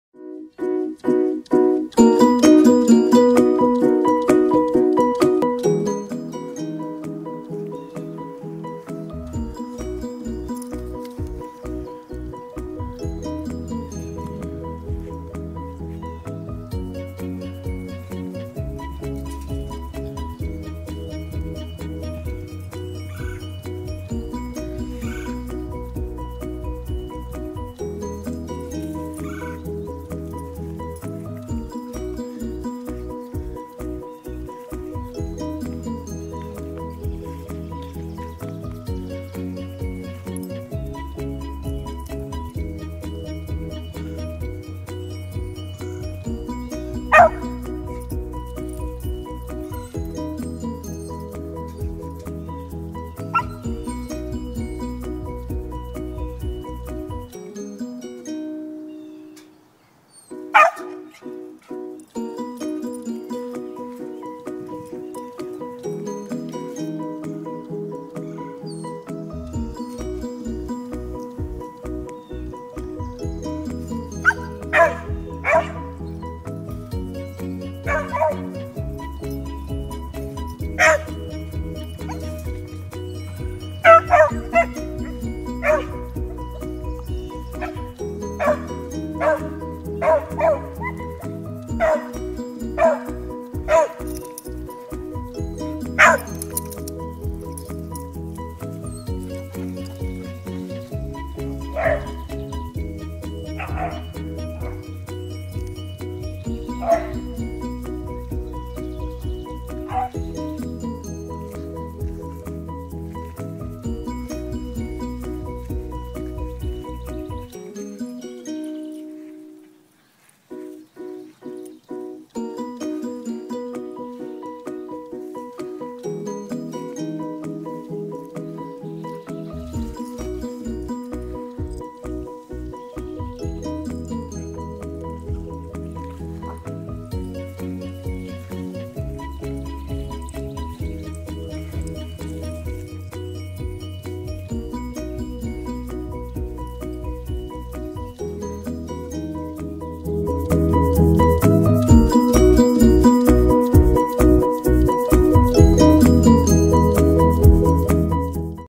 Tiana the Beagle
Tiana is available for adoption with Midwest BREW beagle rescue!  for more information.